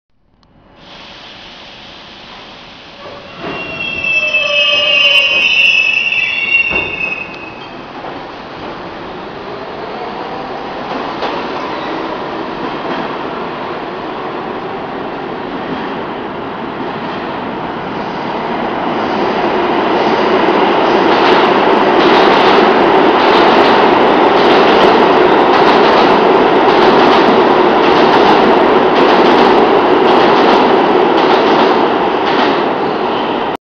R42 4567 V to 2nd Ave & R160A Eto Jamaica Center at 67th Avenue
Recorded in August 2009. 2nd Avenue Bound R42 4567 V at 67th Avenue and Jamaica Center Bound R160A E train .